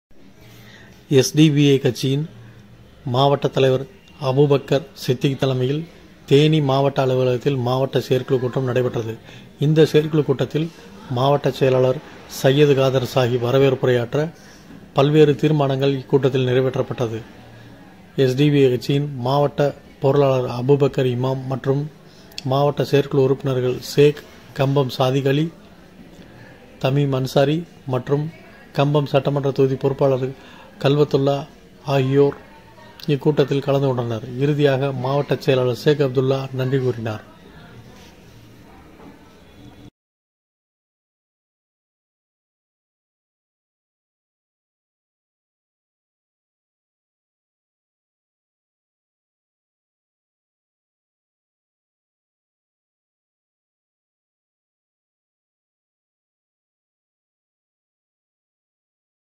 SDPI கட்சி தேனி மாவட்ட செயற்குழு கூட்டம்
SDPI கட்சி மாவட்ட தலைவர் அபுபக்கர் சித்திக்  தலைமையில் தேனி மாவட்ட அலுவலகத்தில் மாவட்ட செயற்குழு கூட்டம் நடைப்பெற்றன. இந்த செயற்குழு கூட்டத்தில் SDPI கட்சி மாவட்ட செயலாளர் சையது காதர் ஷாகிப்  வரவேற்புரையாற்றினார். இக்கூட்டத்தில் பல்வேறு தீர்மானங்கள் நிறைவேற்றப்பட்டன. SDPI கட்சி மாவட்ட பொருளாளர் அபுபக்கர் இமாம்  மற்றும் SDPI மாவட்ட செயற்குழு உறுப்பினர்கள் சேக், கம்பம் சாதிக் அலி, தமீம் அன்சாரி மற்றும் SDPI கட்சி கம்பம் சட்டமன்ற தொகுதி பொறுப்பாளர் கல்வத்துல்லா ஆகியோர் இக்கூட்டத்தில் கலந்து கொண்டனர். இறுதியாக SDPI கட்சி மாவட்ட செயலாளர் சேக் அப்துல்லா  நன்றியுரையாற்றினார்